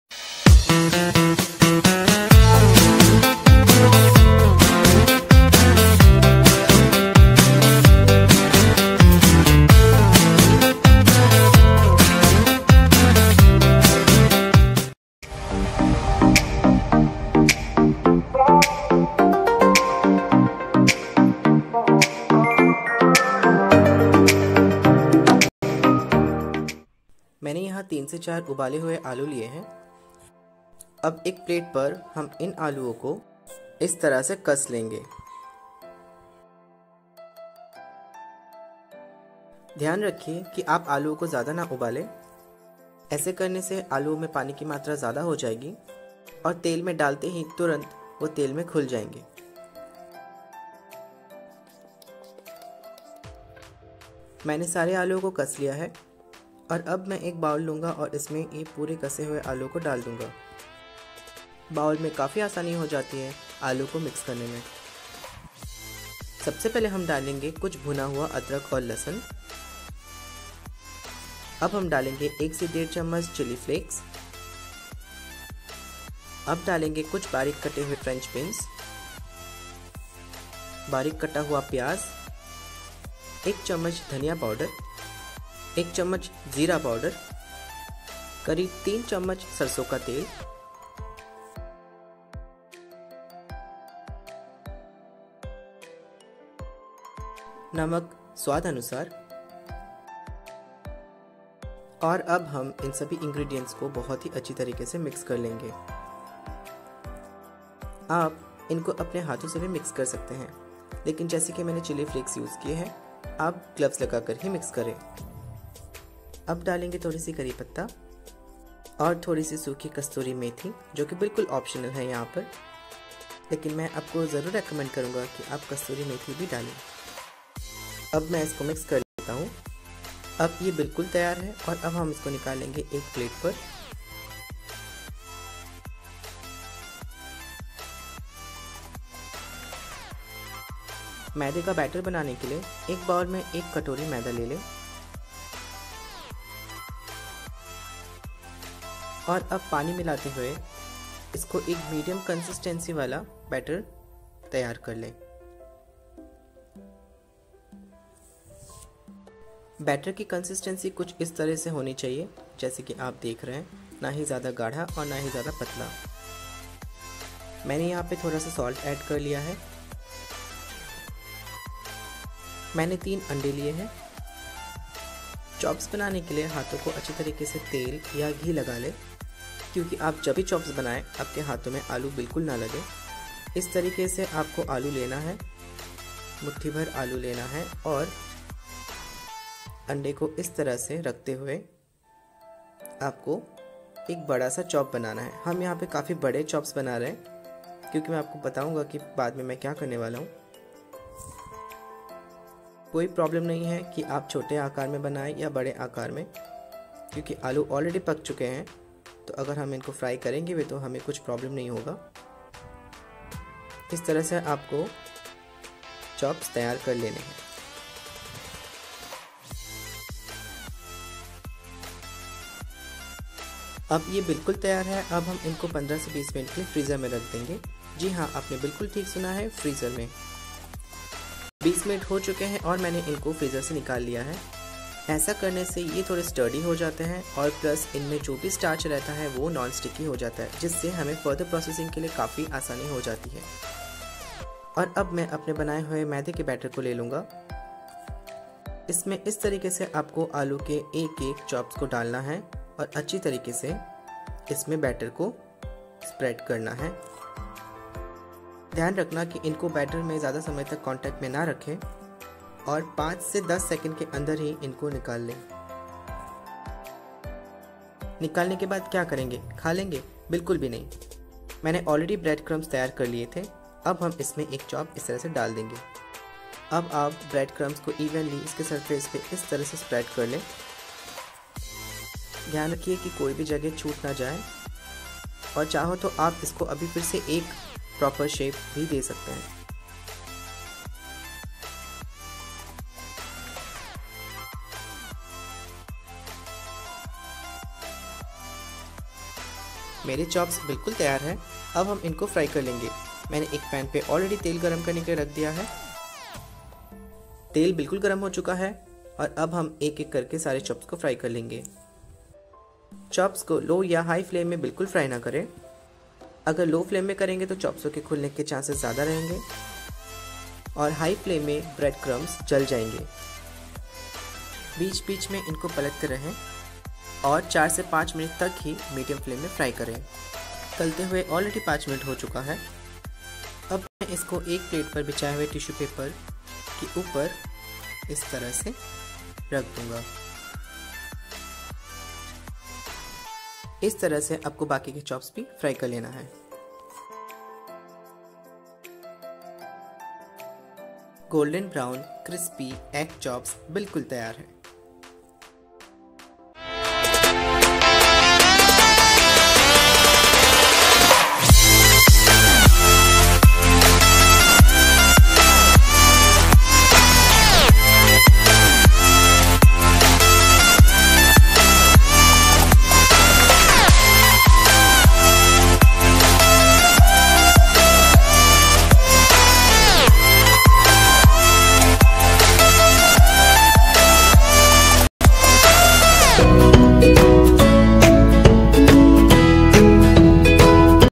How to make Crispy Egg Aloo Chop/Cutlet | Home made | आसान अंडा आलू कटलेट कैसे बनाएं?
Dear Friends,

I know, during holidays you must be searching for something very chatpata to eat..
Try preparing this crispy egg aloo chop at home and trust me you will like it so much for sure.

Thanks
Sandeep Roy Mandal